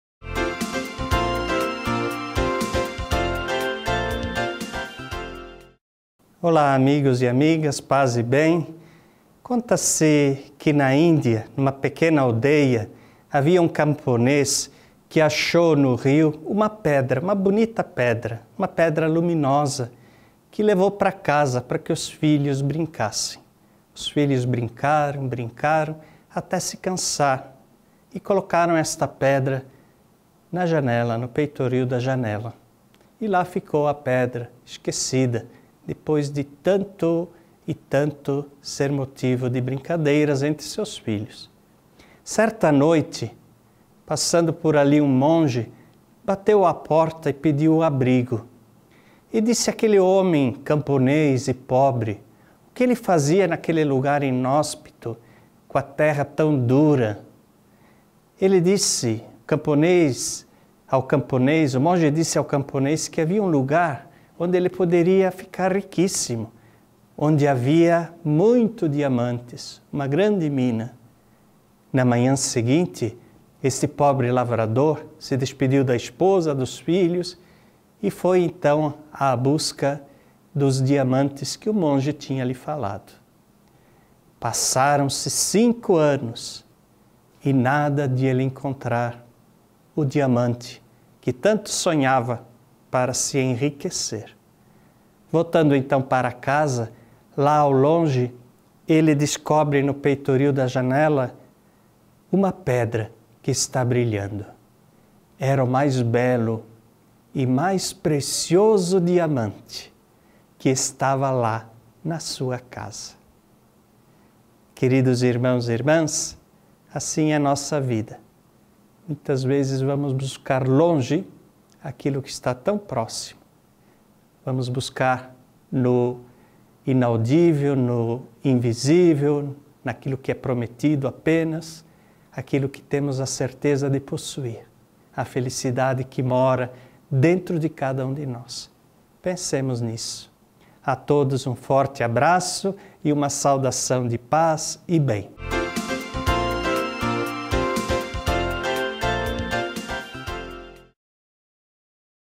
Palavra da Hora - Pedra preciosa
No Palavra da Hora de hoje, Frei Aladim Uber conta uma bonita fábula.

Relembre conosco!